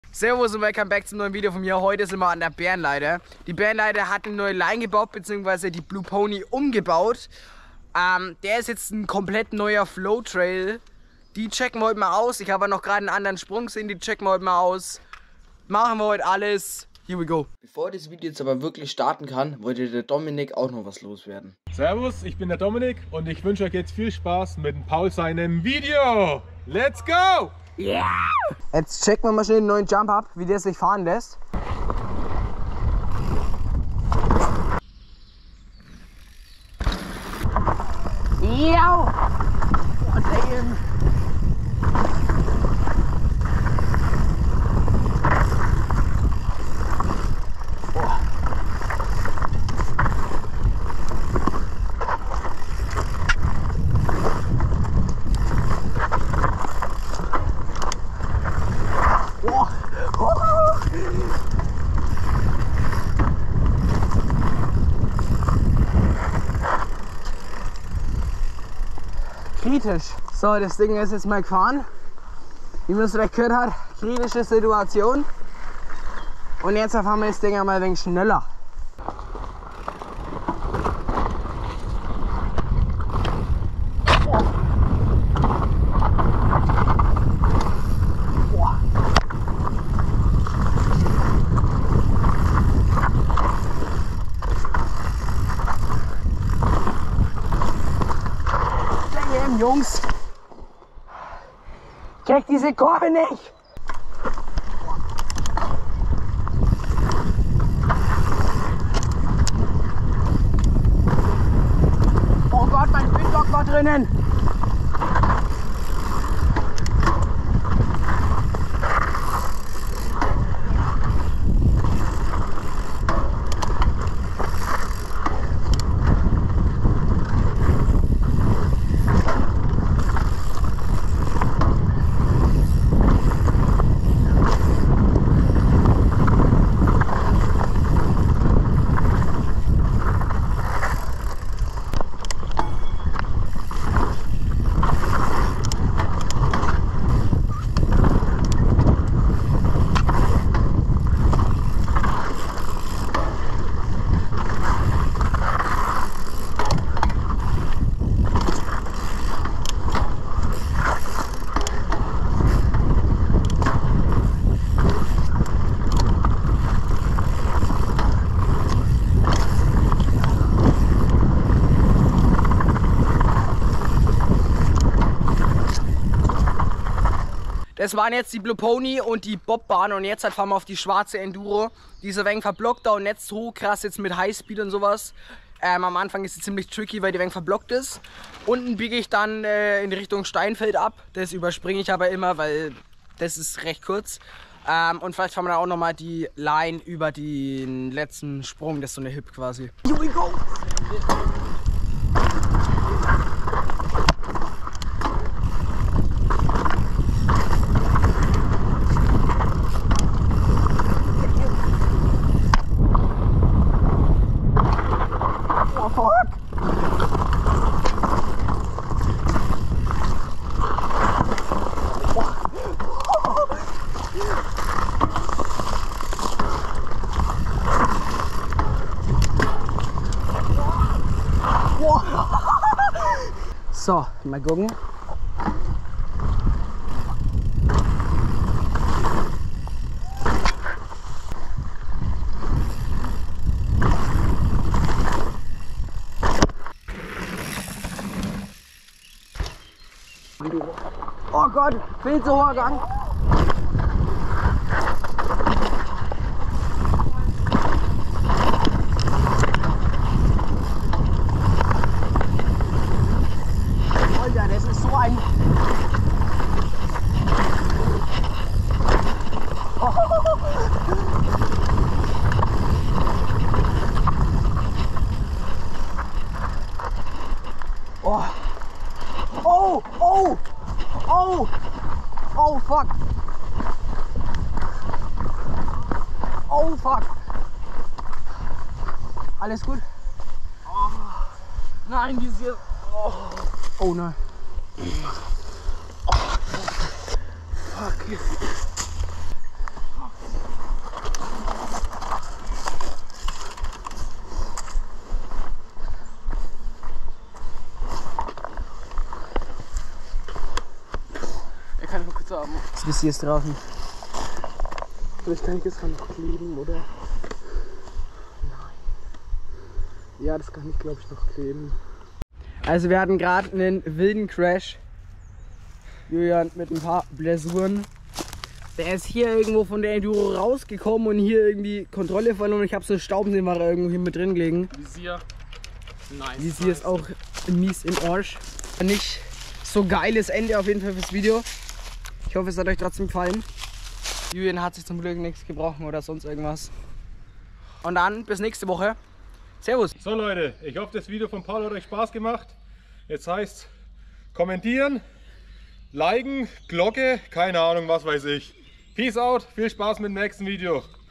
Klein aber fein! / Bärenleitetrails | Scott Ransom 920
Servus
Diese Woche war ich mal wieder an den Bärenleite Trails in Bayreuth. Die locals dort haben gute Arbeit geleistet und die Lines teilweise (Blue Pony komplett) umgebaut. Die Strecken mussten natürlich ausgibt getestet werden.
Viel Spaß beim watch'n
Paul Gröbner

Informationen über das Projekt